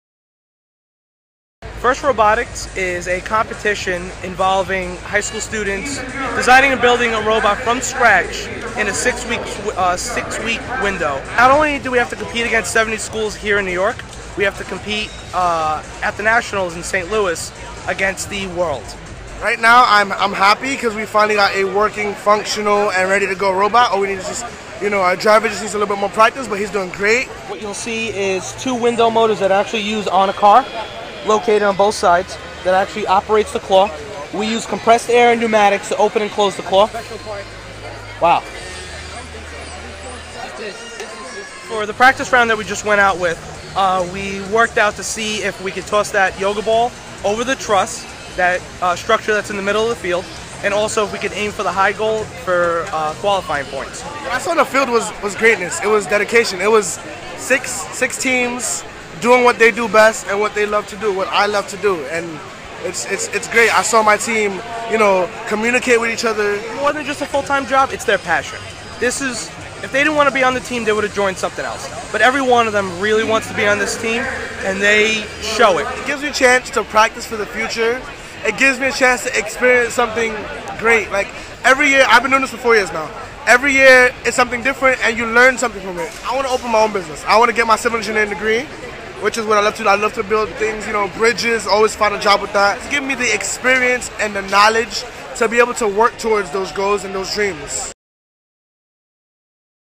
Students Shine at Robotics Competition
NY Daily News shadows the 'Megalodons' of John Dewey High School in Brooklyn as they compete in the FIRST Robotics Competition.